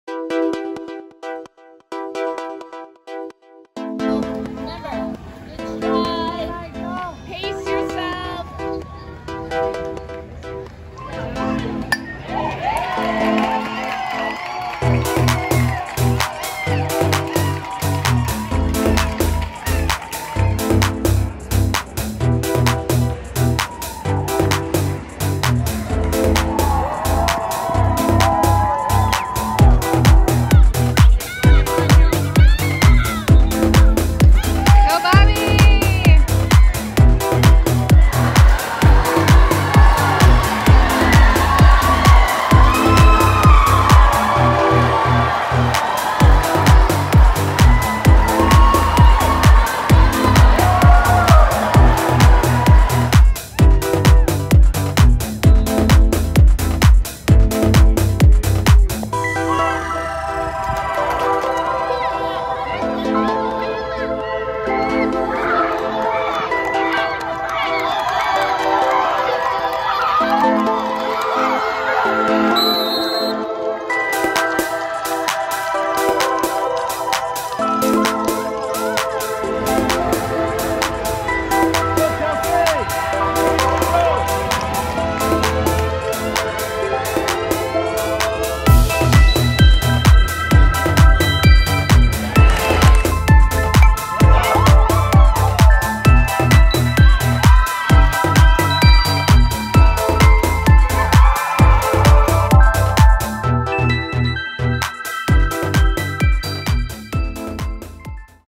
Team Wisconsin 2022 USA Games - Day 1 & 2
Team Wisconsin kicks off 2022 USA Games on Orlando, Fla.